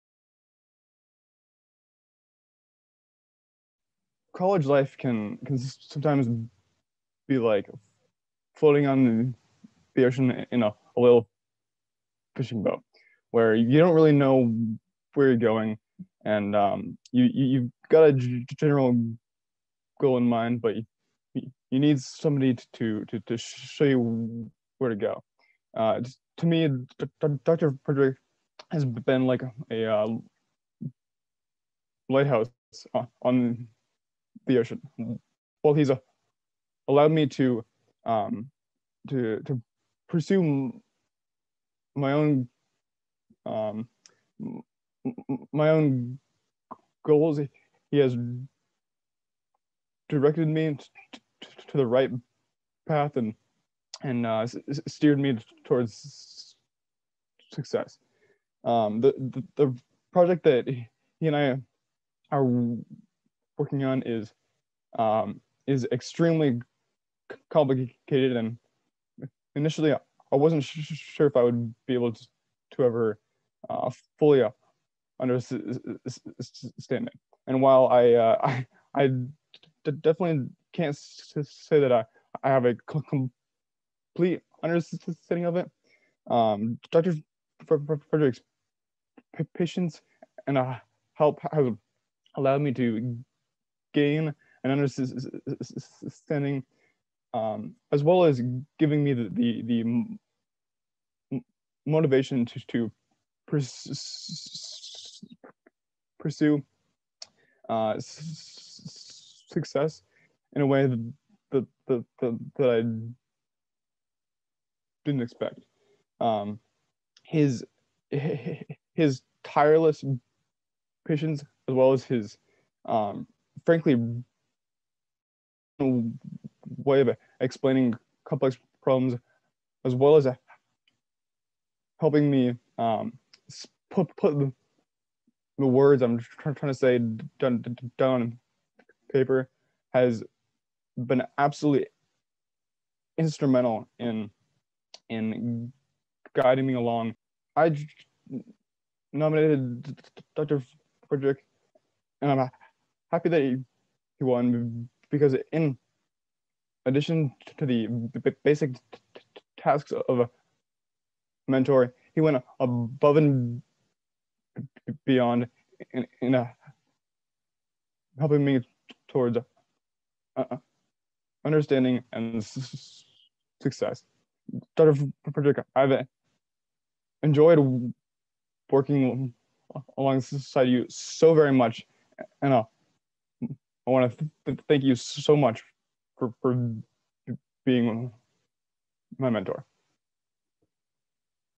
MIW 2021 Faculty Mentor Impact Awards - Brian Frederick
Earlier this year, we asked students to nominate faculty members who had an important impact on them and the response was incredible.

Through online videos and announcements, we are featuring the faculty members who won 2021 Faculty Mentor Impact Awards.